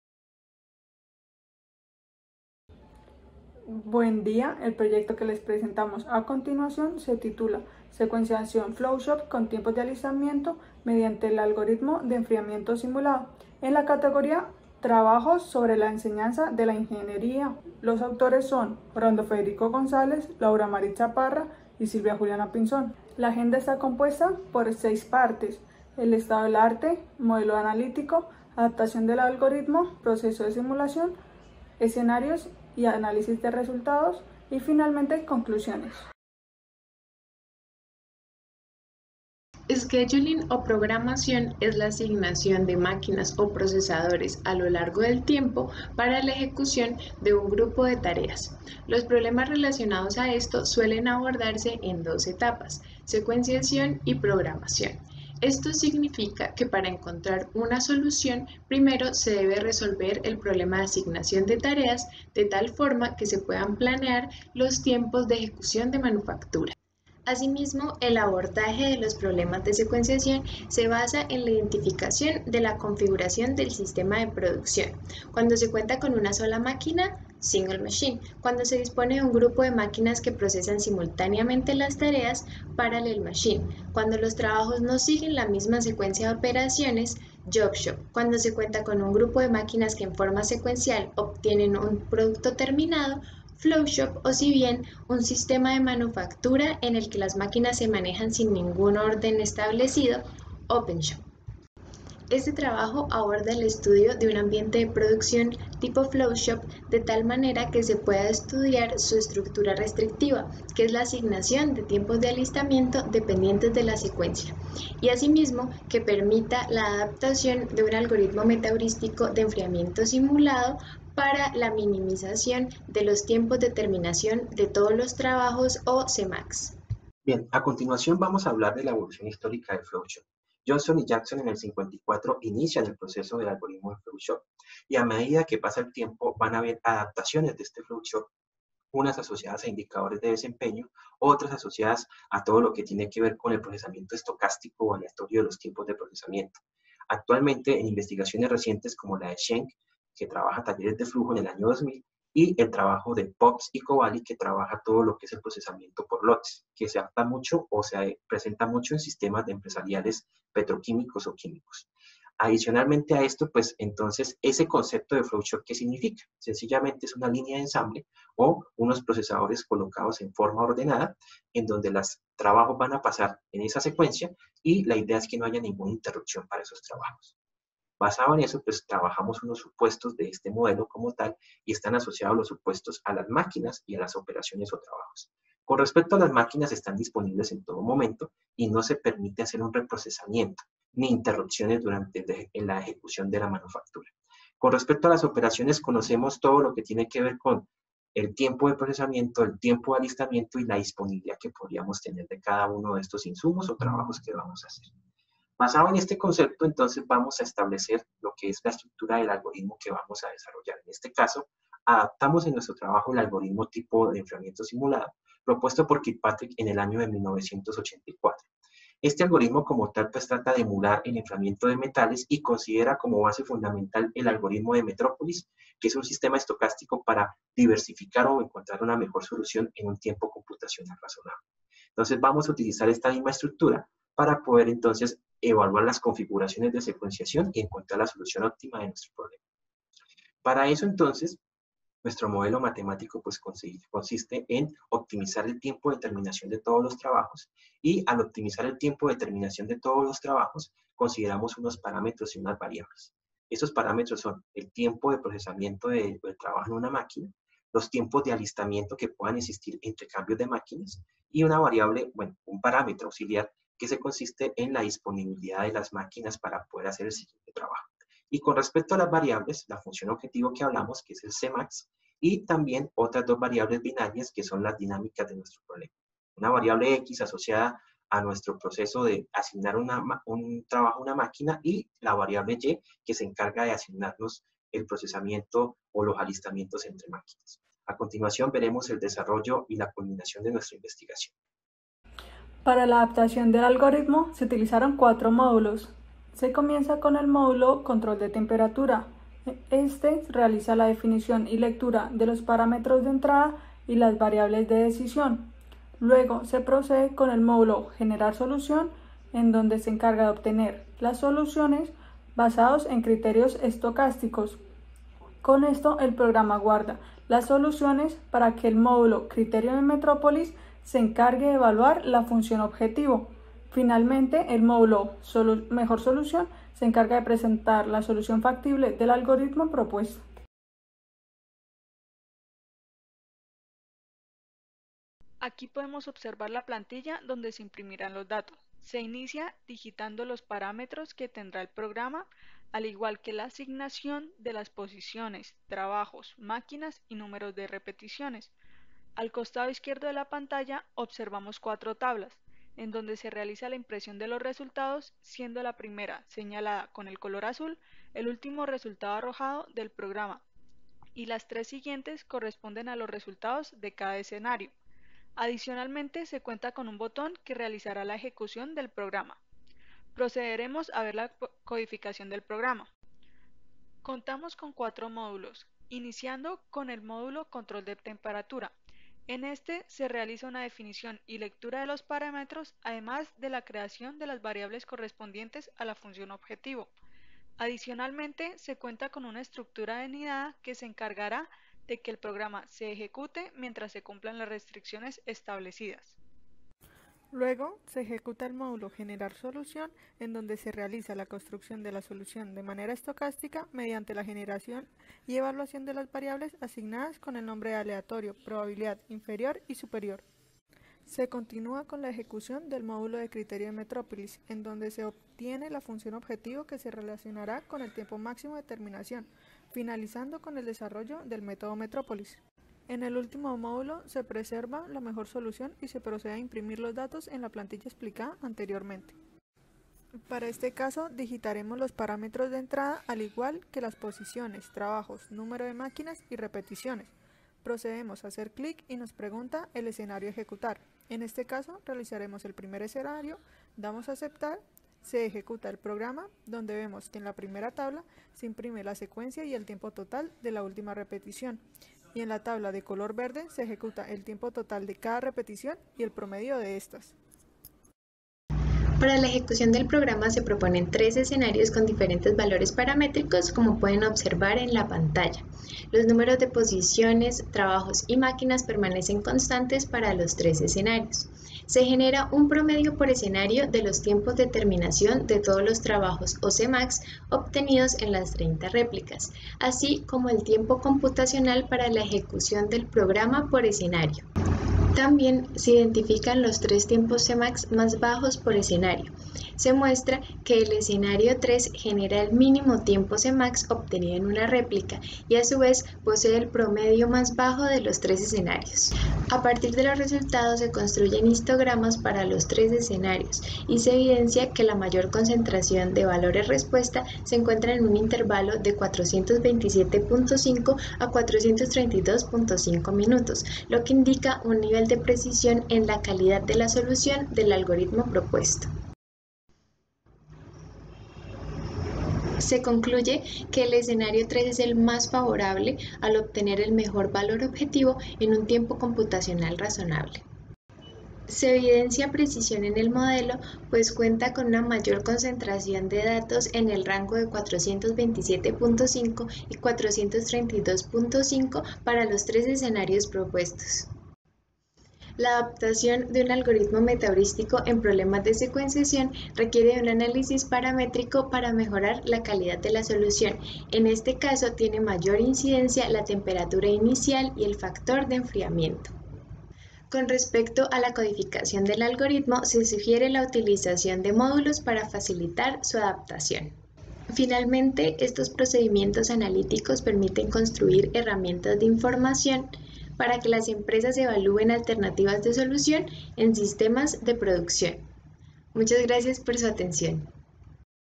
4074
4074. SECUENCIACIÓN FLOW SHOP CON TIEMPOS DE ALISTAMIENTO MEDIANTE ALGORITMO DE ENFRIAMIENTO SIMULADO. Silvia Juliana Pinzón Castiblanco, Laura Maritza Parra Gómez. Universidad Pontificia Bolivariana. Colombia.